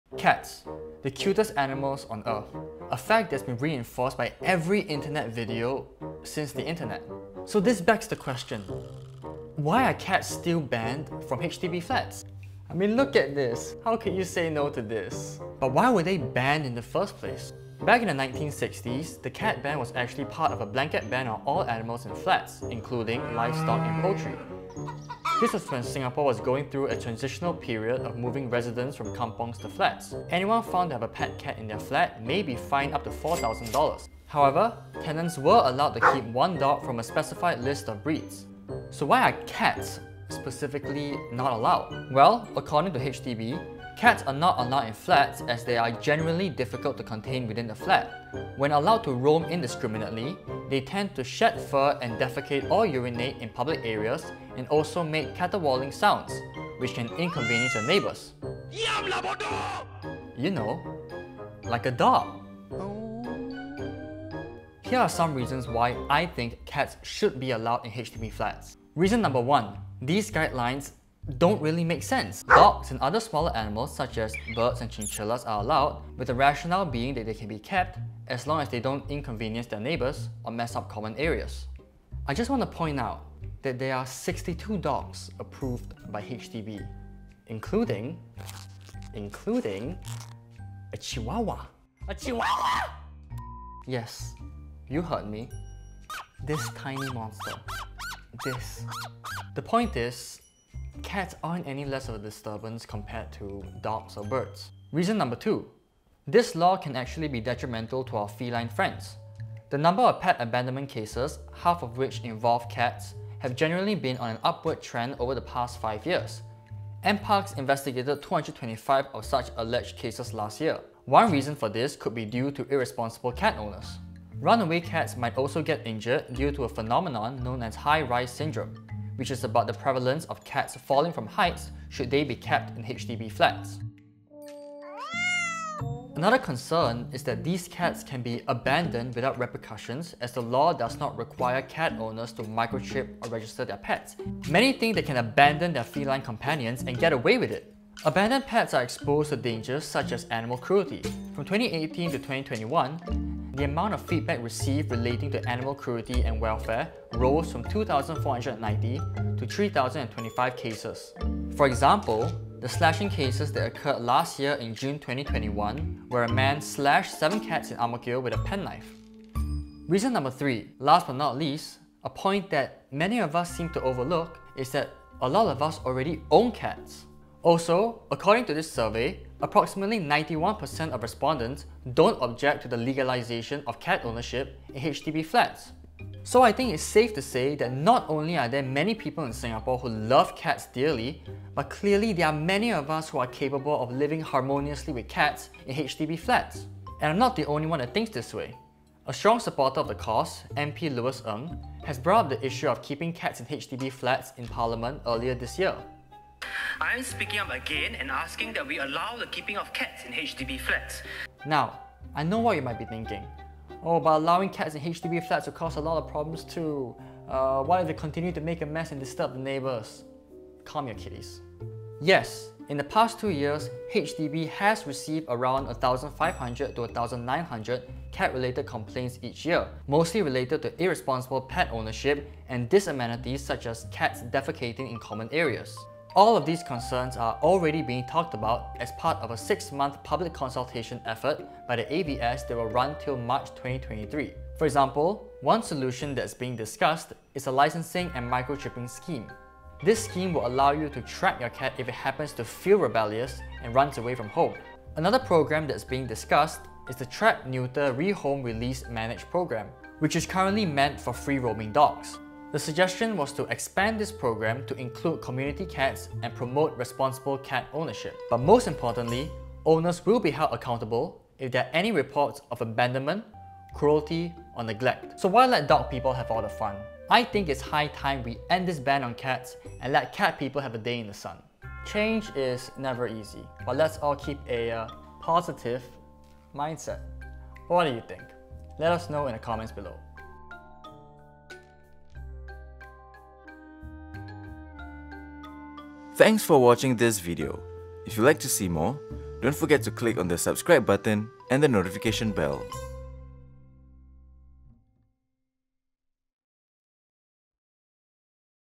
Why are cats banned in Singapore public housing? | MS Explains
Till today, cats are still banned from HDB flats, even though other animals like dogs and birds are allowed. But why?

As a cat would put it, something smells fishy.